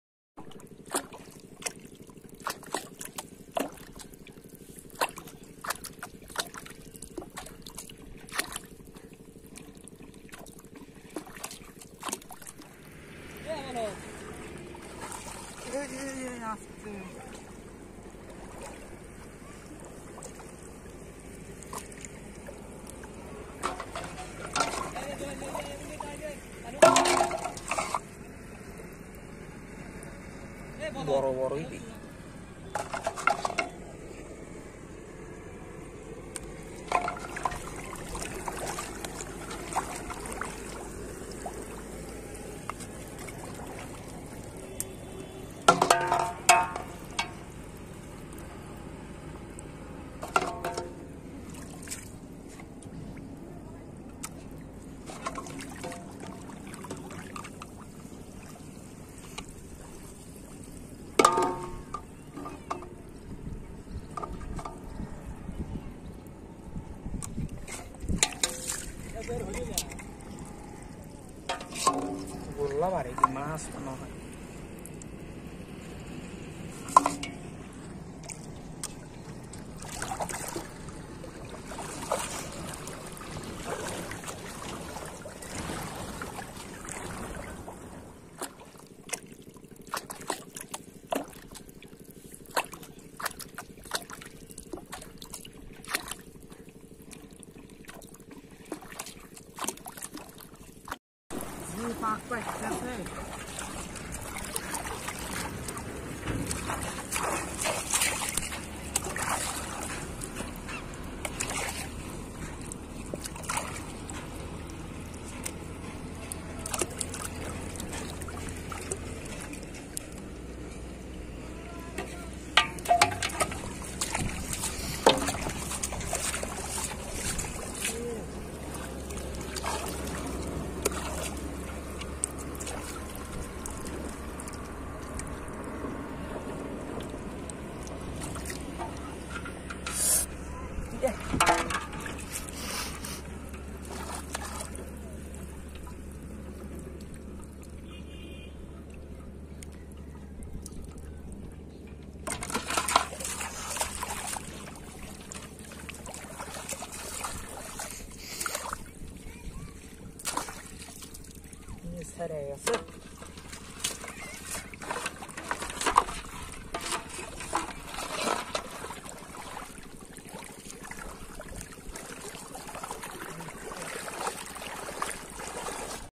Amazing Hook Fishing | Plastic Bottle Fishing Method | Village Boy Catch Fish By Bottle & Hook
Special note  : We never use fake thumbnail in our all videos